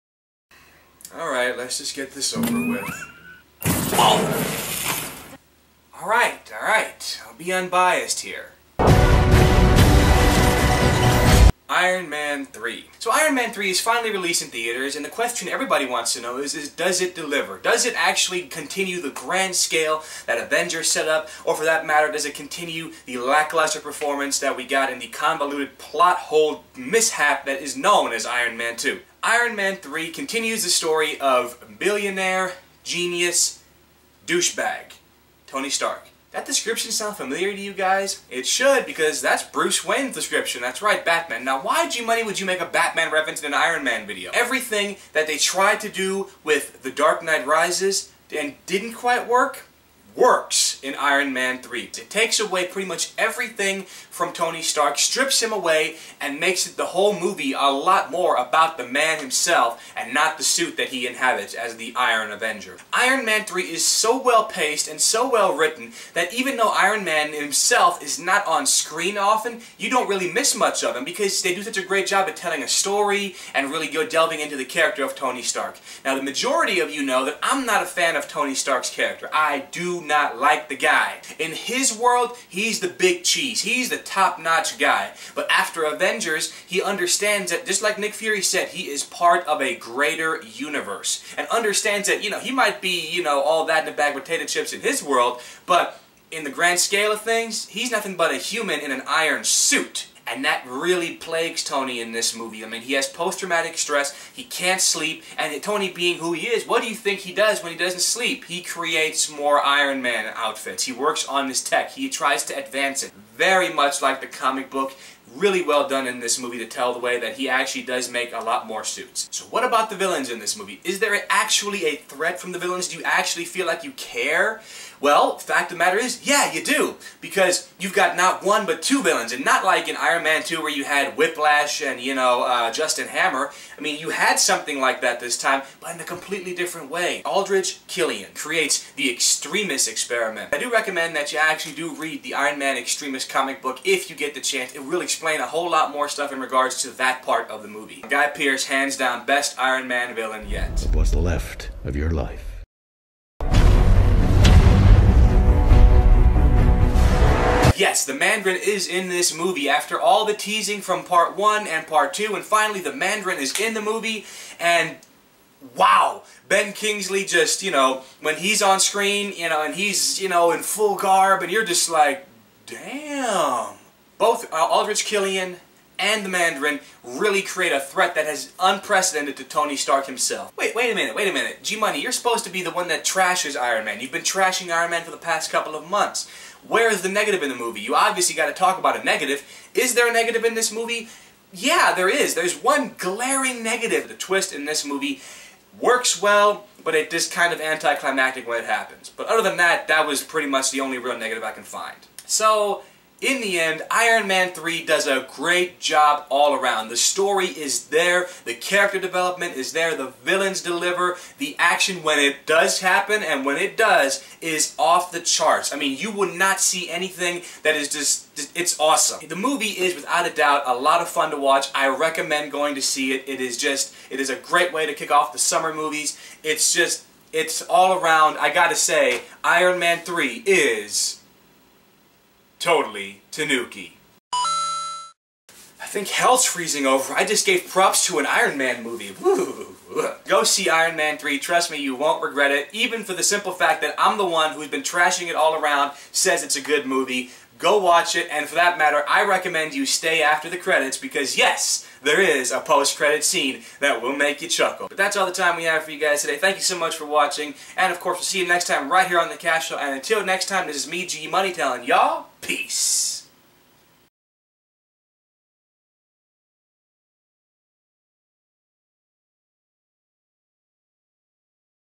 Iron Man 3 review
G-Money checks out if the Iron Avenger delivers in his new feature.